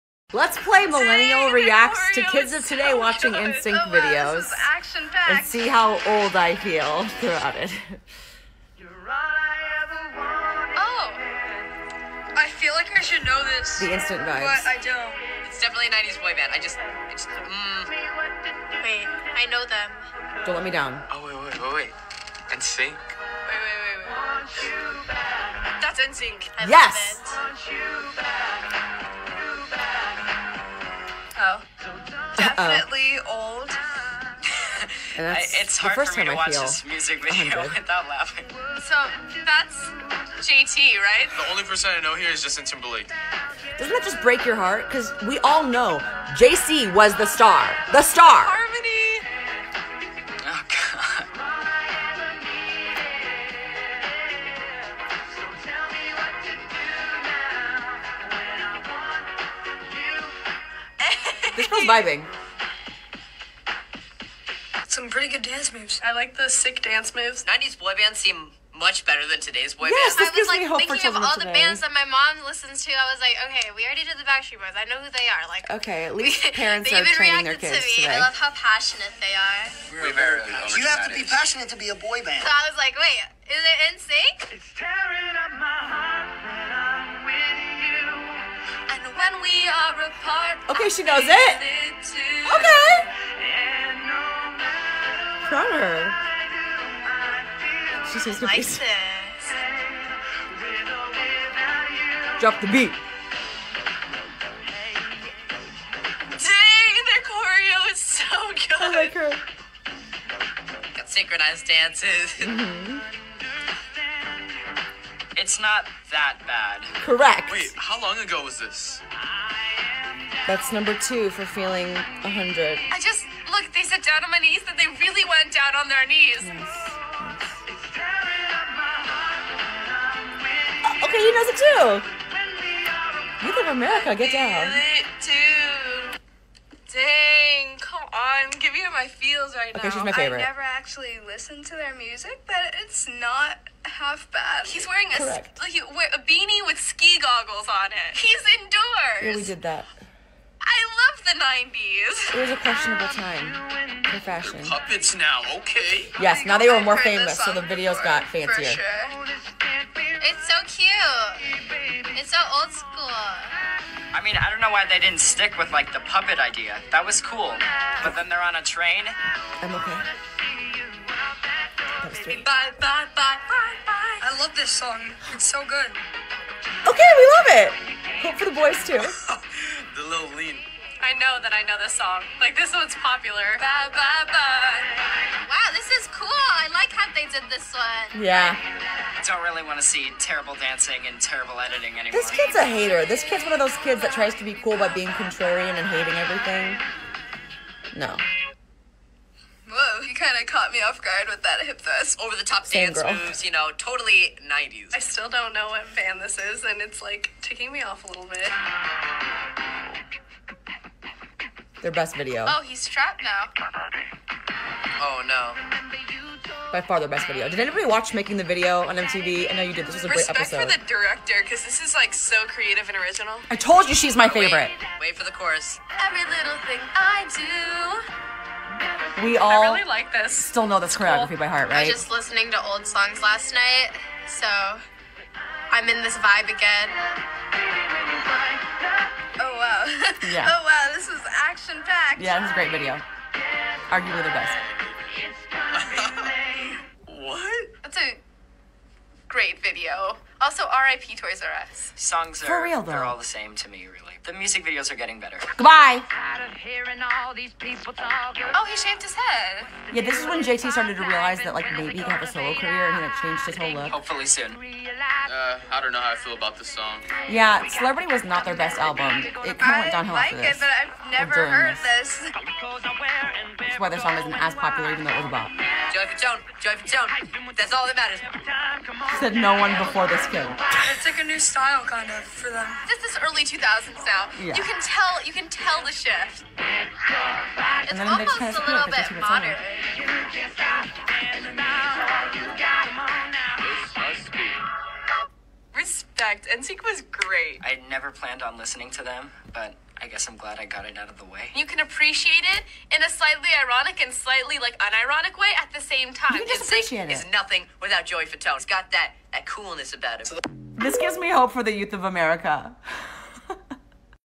Millennial reacts to the youth of today reacting to Nsync music videos…oh the nostalgia! Best boy ba
Millennial reacts to the youth of today reacting to Nsync music videos…oh the nostalgia! Best boy band of all time!